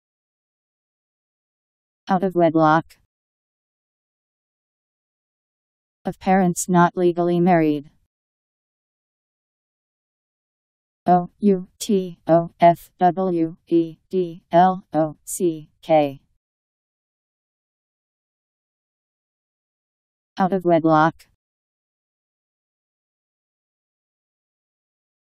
Out of wedlock Meaning
Video shows what out of wedlock means. of parents not legally married.  Out of wedlock Meaning. How to pronounce, definition audio dictionary. How to say out of wedlock. Powered by MaryTTS, Wiktionary